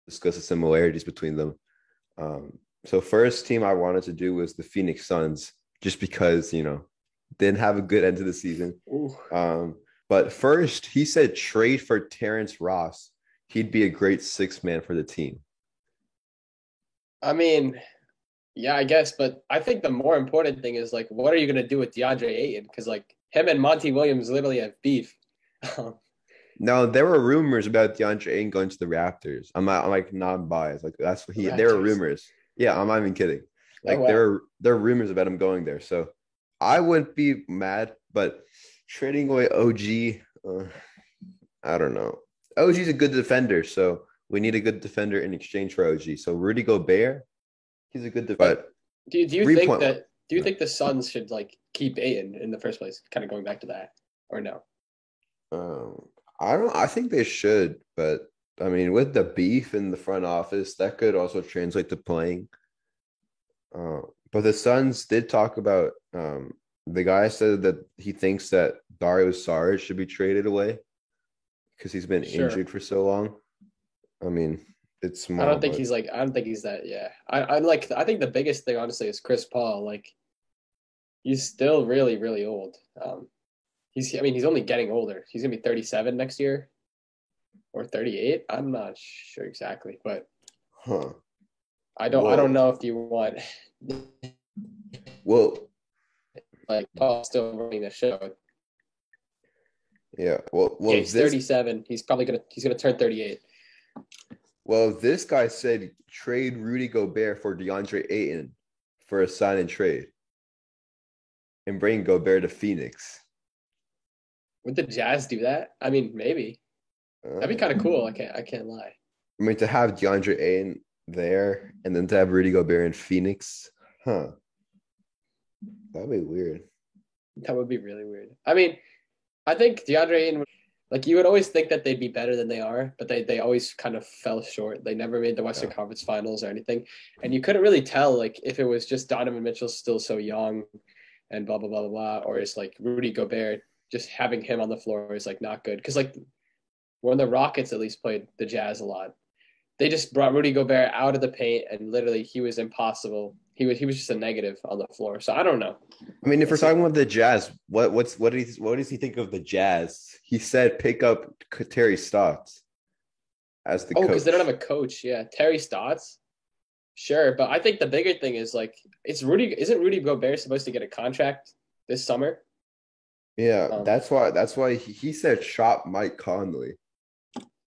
The Phoenix Suns Get WHO?
Daniel and Ekamresh share their predictions on the Phoenix Suns during this upcoming offseason. They talk about what trades they are going to make, and if Ayton’s relationship with the front office is ruined.